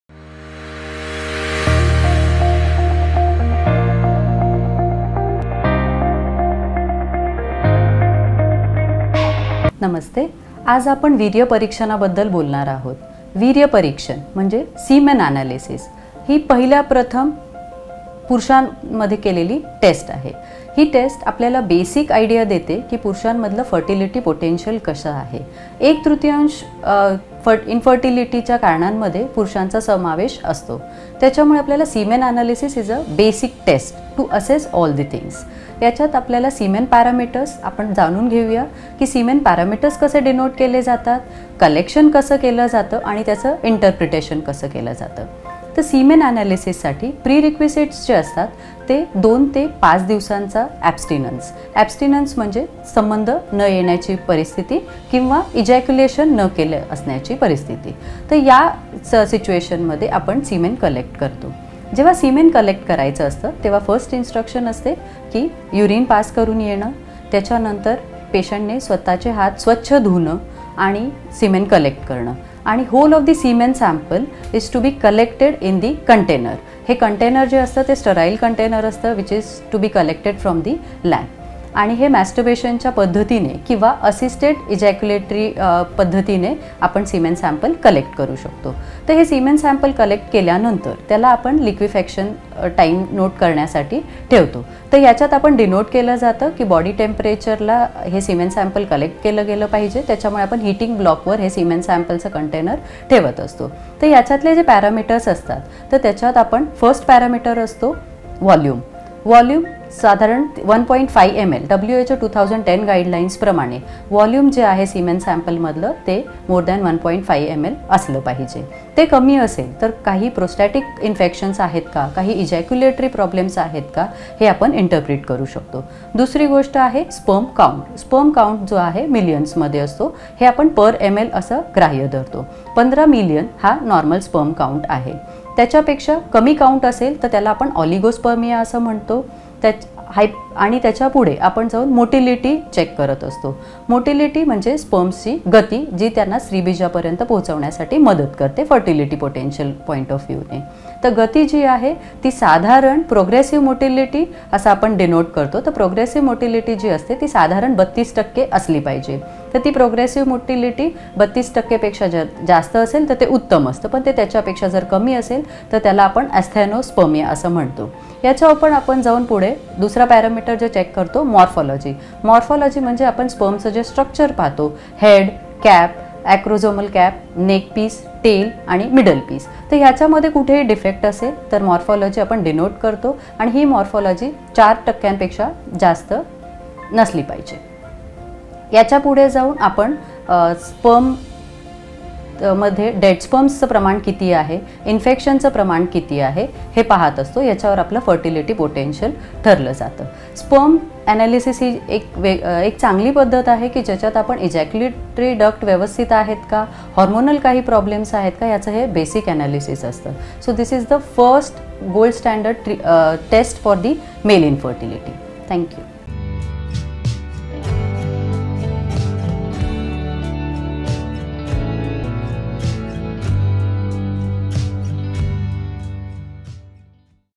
What are the Parameters of a Semen Analysis?| Dr Nisha Pansare |Fertility Specialist | Nova IVF-Pune
Dr Nisha Pansare, Fertility Specialist, Nova IVF- Pune talks about-
What are the Parameters of a Semen Analysis?
Semen analysis is the test performed on males to determine their basic fertility potential, as males account for approximately one-third of all cases of infertility. That's why semen analysis is a basic test to assess all these things.

The parameters seen in this section are how semen parameters are denoted, how collection is done, and how it is interpreted. Abstinence for 2-3 days is required for sperm analysis.

Abstinence means the condition of having no intercourse or having no ejaculation. In this situation, semen is collected. When sperm must be collected, the first instruction is that one should pass urine, wash their hands, and then collect the sperm.

The whole semen sample should be collected in a container that is sterile, which is to be collected from the lab. It can be collected through masturbation or assisted ejaculatory methods. After the collection of semen, it is kept to note the liquefaction time. So in this, it is indicated that semen samples should be collected at body temperature; that’s why the container is kept on the heating block.

The parameters that are included in this are: first, volume, which should be 1.5 mL as per WHO guidelines; volume should be more than 1.5 mL. It can rule out prostatic infection or ejaculatory problems if it is low. The second factor is sperm count, which is measured in millions and calculated per millilitre. 15 million/mL is a normal sperm count; if it is lower, it is called oligospermia.

After that, motility is checked; motility means the speed of the sperm, which helps them reach the egg from a fertility potential point of view, so normal speed is denoted as progressive motility; progressive motility should be 32%. If it is higher, that is excellent; if it is lower, that is asthenospermia.

After this parameter, morphology is checked, which means the structure of the sperm head, cap, acromial cap, neck piece, tail, and middle piece. So, if there is a defect, the morphology is denoted, and it should not be more than 4%; after that, the number of dead sperms is checked, as well as the presence of infection. From here, the fertility potential is defined.

Sperm analysis is a very good method by which one can examine the condition of the ejaculatory duct and normal problems. It is the basic analysis, so this is the first gold standard test for male infertility.

0:00 Introduction
0:13 What are the Parameters of a Semen Analysis?
1:01 Abstinence
1:58 Semen Volume
2:30 Semen motility
2:59 Morphology